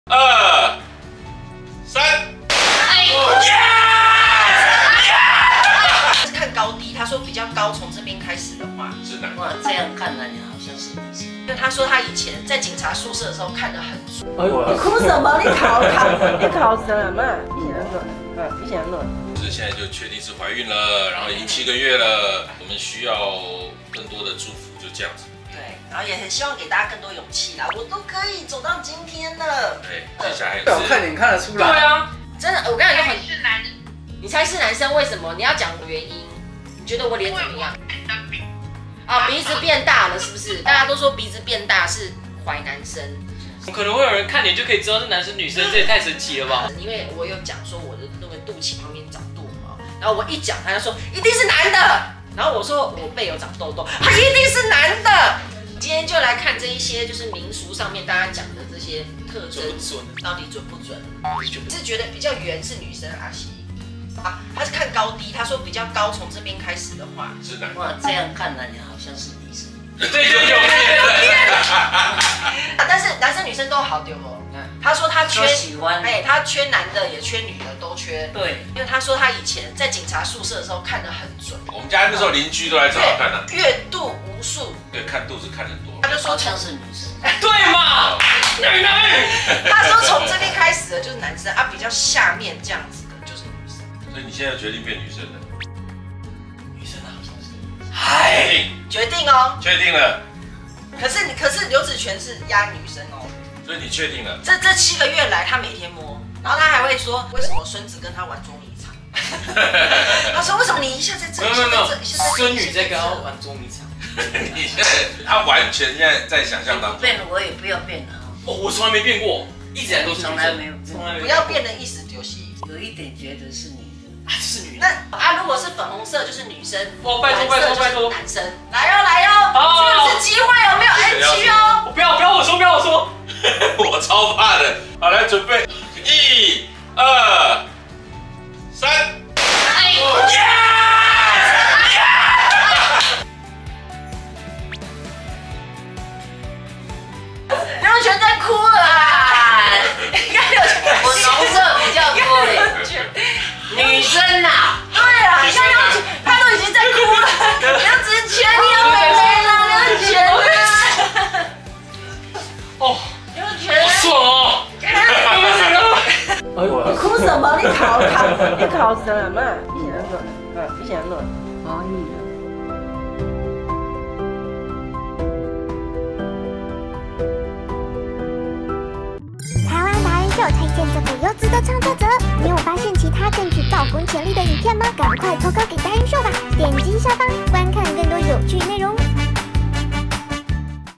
【趙小僑懷孕開趴猜寶寶性別 婆婆摸30周肚看高低猜女生】｜@ChaoHsiaoChiao
看的好感動...😭😭恭喜~~~(泰泰編)